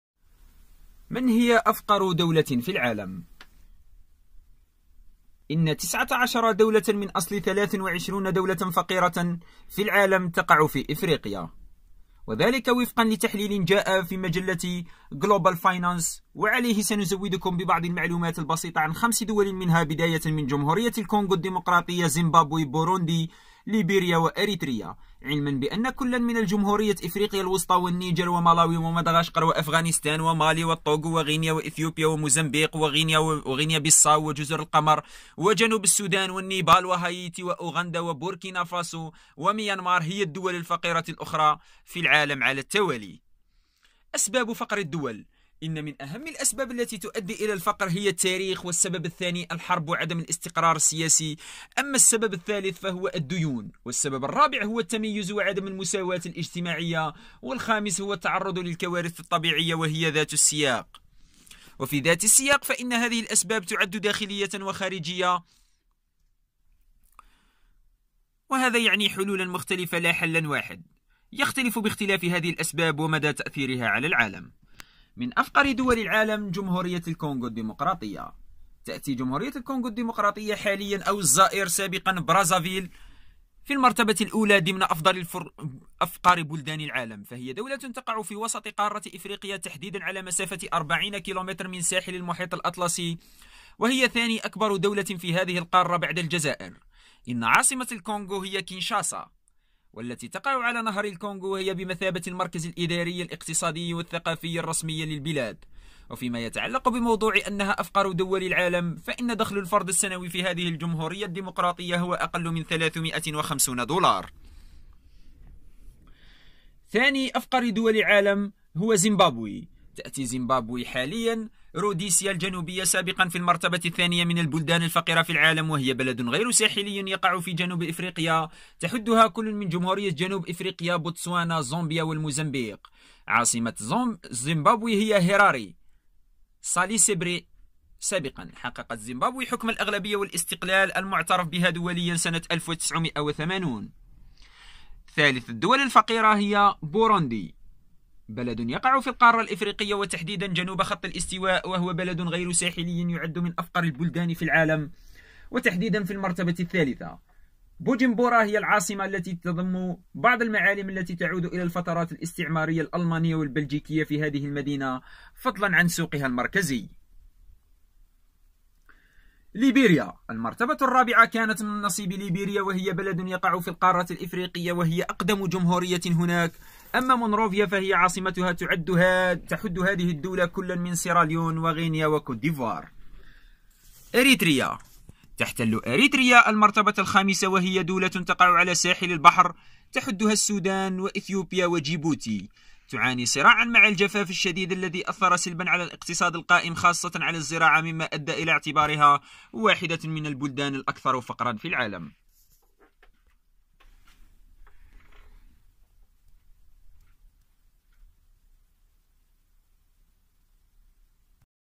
أفقر دولة في العالم
إن 19 دولة من أصل 23 دولة فقيرة تقع في إفريقيا وذلك وفقاً لتحليل جاء في مجلة غلوبال فاينانس، وعليه سنزودكم ببعض المعلومات البسيطة عن خمس دول منها، بداية من: جمهورية الكونغو الديموقراطية، وزيمبابوي، وبوروندي، وليبيريا وإريتريا، علماً بأن كلاً من جمهورية إفريقيا الوسطى، والنيجر، ومالاوي، ومدغشقر، وأفغانستان، ومالي، وتوجو، وغينيا، وإثيوبيا، وموزمبيق، وغينيا- بيساو، وجزر القمر، وجنوب السودان، ونيبال، وهاييتي، وأوغندا، وبوركينا فاسو، وميانمار هي الدول الفقيرة الأخرى في العالم على التوالي. Volume 0%